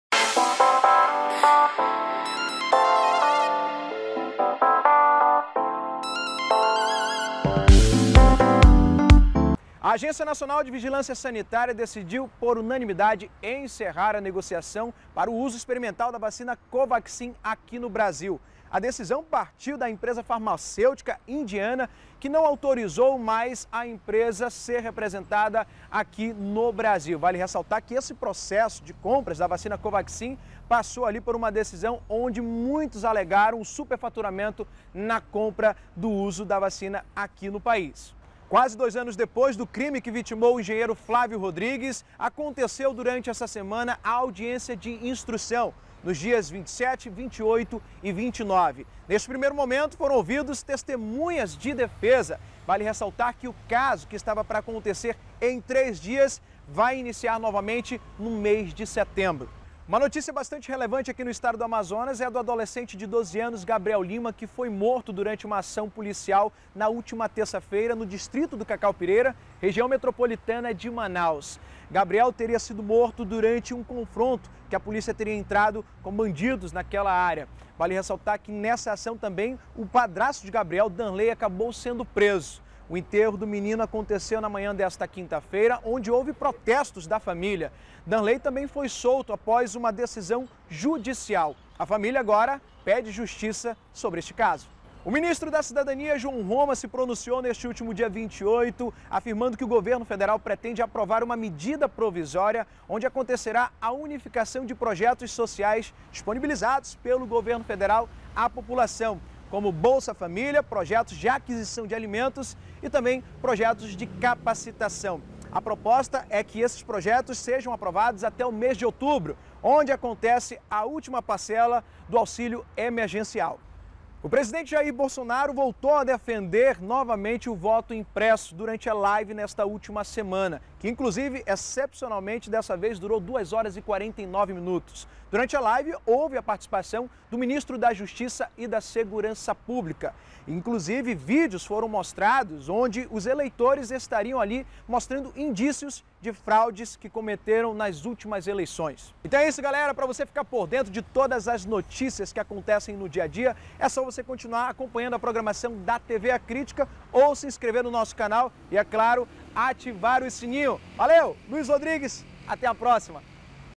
TOP 5 NOTÍCIAS: QUEM MATOU O ENGENHEIRO FLÁVIO E POR QUÊ? #3
Confira os principais destaques do que foi notícia na semana do dia 25/07/21 a 31/07/21.

Repórter: Luiz Rodrigues
Produção: Vinícius Xavier
Edição de texto: Vinícius Xavier
Cinegrafista: Mário Barbosa
Edição de imagem: Iron Michel